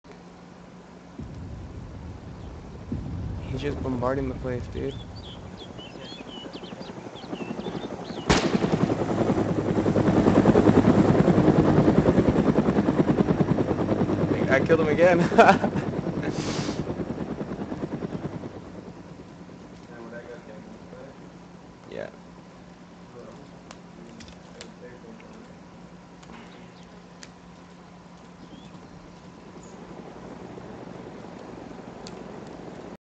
Messiah's - Moving 750m+ Sniper Kill
Yes you can say using a thermal scope is ez and noob...
but its in this server and only sniper i found! so suck it!
doesnt change the fact its a 750+ meter kill shot while moving :D